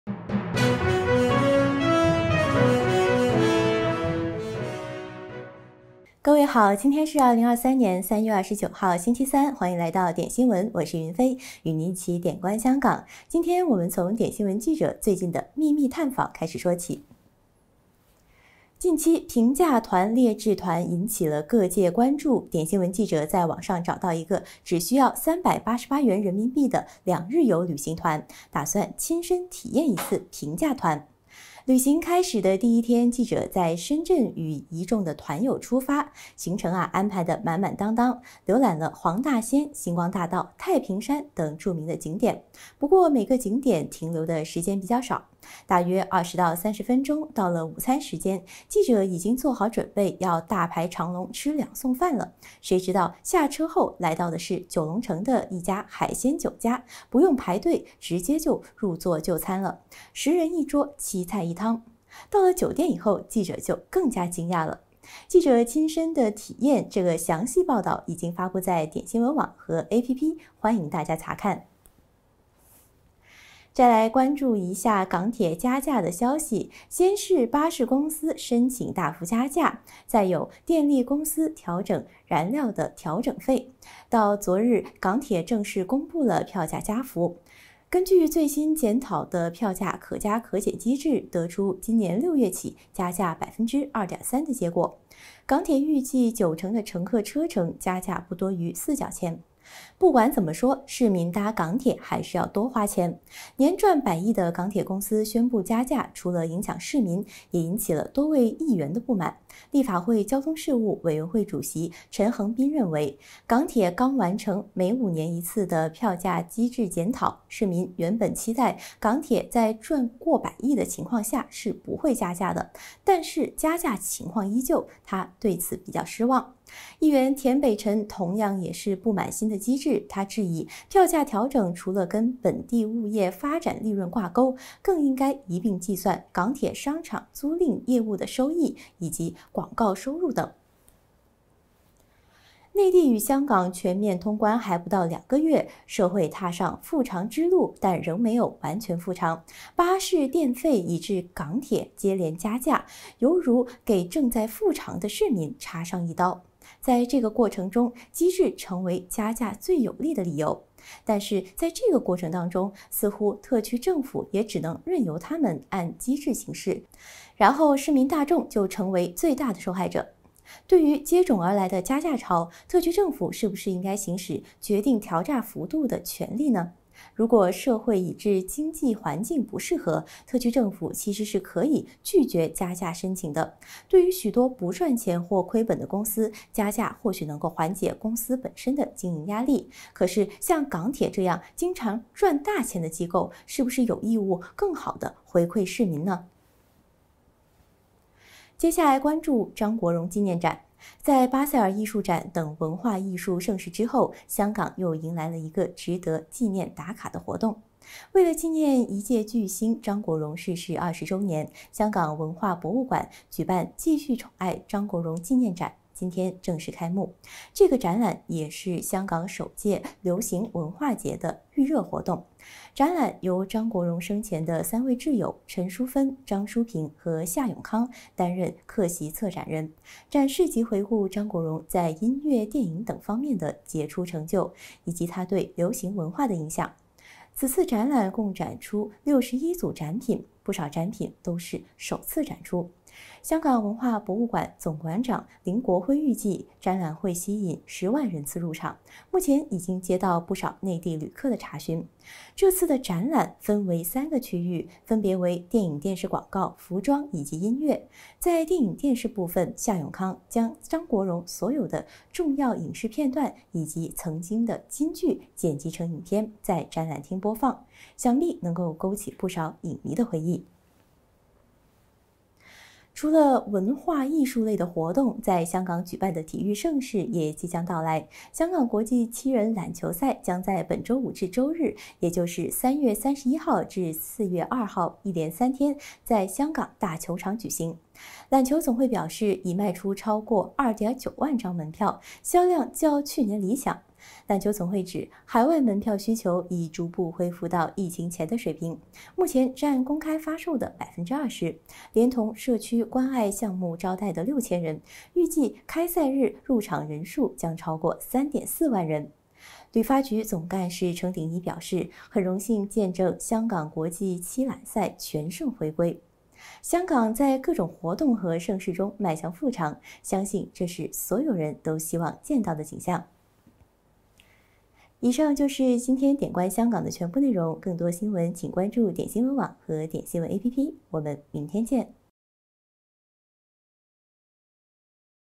點觀香港｜記者親試「低價團」真相驚人 張國榮紀念展重溫經典
近期，「平價團」、「劣質團」引起各界關注。點新聞記者在網上找到一個只需要388元人民幣的「兩日遊」旅行團，打算親身體驗一次平價團。旅遊開始的第一天，記者在深圳與一眾團友出發，行程安排得滿滿當當，瀏覽了黃大仙、星光大道、太平山等著名景點，不過每個景點停留時間較少，大約20到30分鐘。到了午餐時間，記者已經做好準備要大排長龍吃「兩餸飯」了，誰知下車後來到的是九龍城的一間海鮮酒家，不用排隊直接入座就餐，10人一桌，7菜1湯。到了酒店後記者就更加驚訝了。記者親身體驗的詳細報道已經發布在點新聞網站和點新聞App，歡迎大家查看。（詳情點擊：
再來關注港鐵加價的消息。先是有巴士公司申請大幅加價，再有電力公司調整燃料調整費，到昨日，港鐵公布了票價加幅，根據最新檢討的票價「可加可減」機制，得出今年6月起加價2.3%的結果。年賺百億的港鐵公司宣布加價，讓市民叫苦，也引起了多位議員的不滿，立法會交通事務委員會主席陳恒鑌認為，港鐵剛完成每5年一次的票價機制檢討，市民原本期待港鐵在賺過百億元之下不會加價，但是加價情況依舊，他對此感到失望。議員田北辰同樣不滿新機制，他質疑票價調整除了與本地物業發展利潤掛鈎，更應該一併計算港鐵商場租務收益及廣告收入等。
內地與香港全面通關還不足兩個月，社會踏上復常之路但仍未完全復常，巴士、電費以至港鐵接連加價，猶如向正在「康復」的市民插上一刀，在這過程中，機制成為了加價最有利的理由，但這過程中，似乎特區政府也只能任由它們按機制行事，市民大眾成為最大的受害者。對於接踵而來的加價潮，特區政府是不是應該行使有決定調價幅度的最終裁決權呢，如果社會以至經濟環境不合適，特區政府其實是可以拒絕加價申請的。對於許多不賺錢或虧本的公司，加價或許能夠緩解本身的經營壓力，可是像港鐵這些經常賺大錢的機構，是不是更有義務更好地回饋市民呢。
接下來關注張國榮紀念展。在巴塞爾藝術展等文化藝術盛事之後，香港又迎來了一系列值得前來打卡的活動！為紀念一代巨星張國榮逝世20周年，香港文化博物館舉辦「繼續寵愛·張國榮紀念展」，該展今日正式開幕。這個展覽也是香港首屆流行文化節的預熱活動，展覽由張國榮生前的三位摯友陳淑芬、張叔平及夏永康擔任客席策展人，展示及回顧張國榮在音樂、電影等方面的傑出成就，及其對流行文化的影響。此次展覽共展出61組展品，不少展品都是首次展出。香港文化博物館總館長林國輝預計，展覽會吸引10萬人次入場，目前已接到不少內地遊客的查詢。（詳情點擊：
除了文化藝術類的活動，在香港舉辦的體育盛事也即將到來。香港國際七人欖球賽將在本周五至周日，也就是3月31日至4月2日，一連三日在香港大球場舉行。欖球總會表示，已賣出超過2.9萬張門票，銷量較去年理想。欖球總會指，海外門票需求已逐步回復至疫情前的水平，目前佔公開發售的20%。連同社區關愛項目招待的6000人，預計開賽日入場人數將超過3.4萬人。香港在各種活動和盛事中邁向復常，相信這是所有人都希望見到的景象。
記得每周一到周五和我們一起，觀香港、看世界。